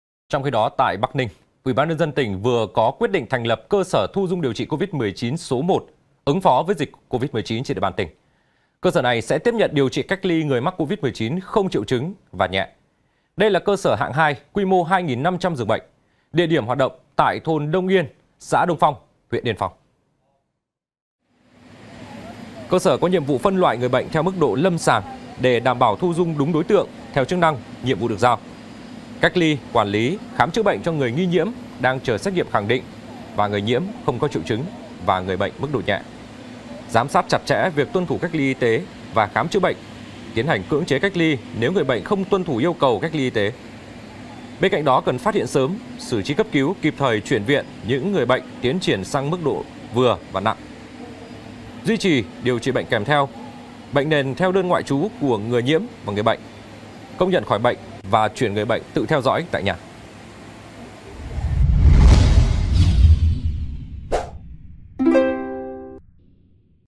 Bắc Ninh thành lập cơ sở thu dung, điều trị Covid-19 số 1| VTC14
VTC14 |Trong khi đó, tại Bắc Ninh, UBND tỉnh vừa có quyết định thành lập Cơ sở thu dung điều trị COVID-19 số 1 ứng phó với dịch bệnh COVID-19 trên địa bàn tỉnh. Cơ sở này sẽ tiếp nhận điều trị, cách ly người mắc COVID-19 không triệu chứng và nhẹ. Đây là cơ sở hạng II, quy mô 2.500 giường. Địa điểm hoạt động tại thôn Đông Yên, xã Đông Phong, huyện Yên Phong.